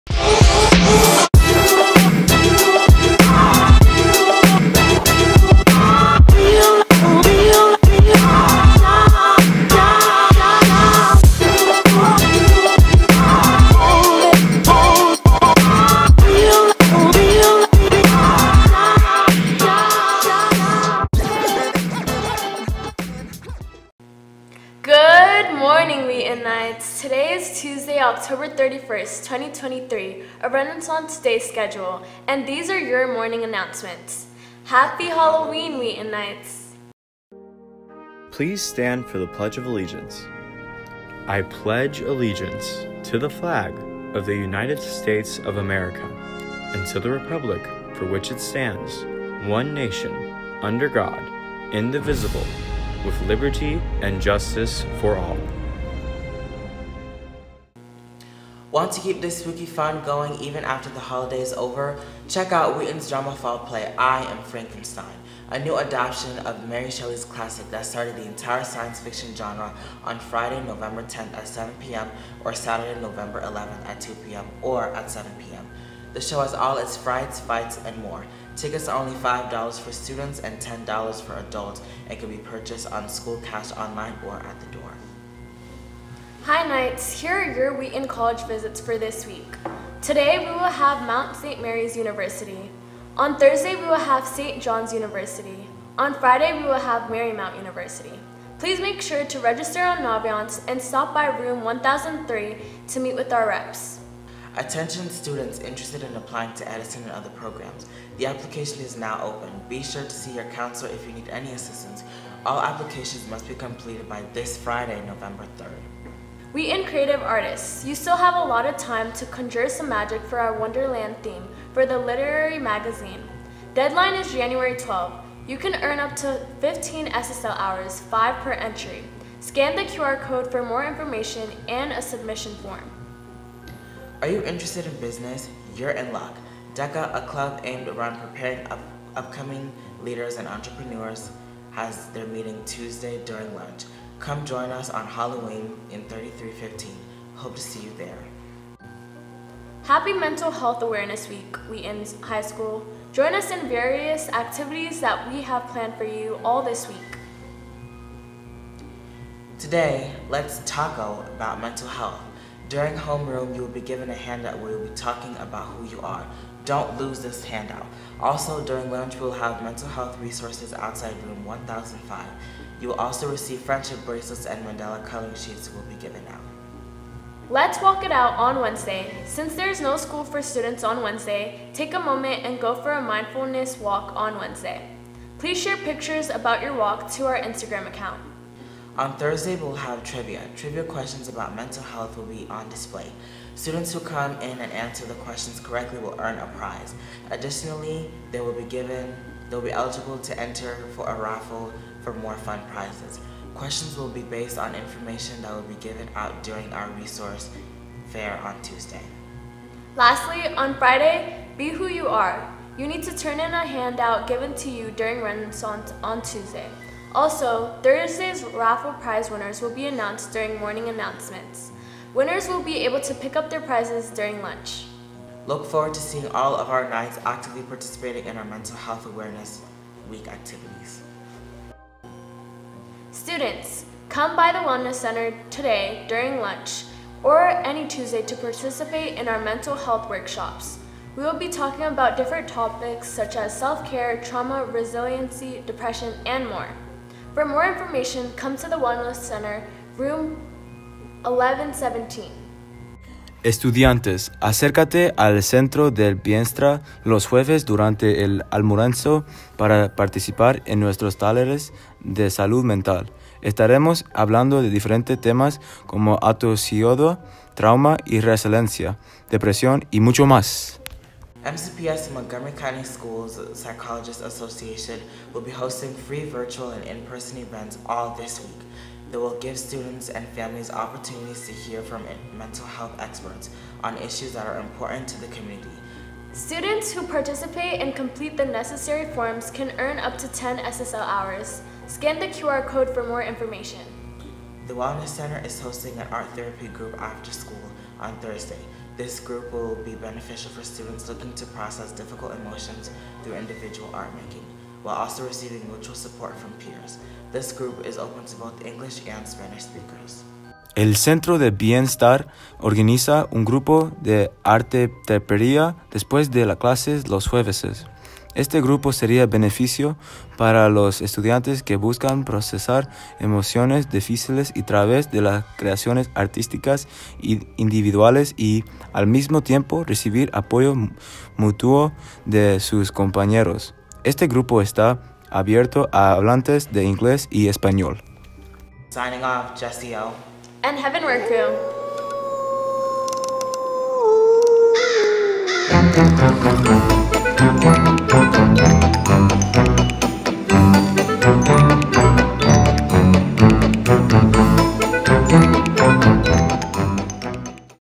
October 31 2023
Wheaton HS Morning Announcements for Halloween, 2023. As this is also Mental Health Awareness Week, today's announcements have a lot of information about resources for Mental Health available throughout the week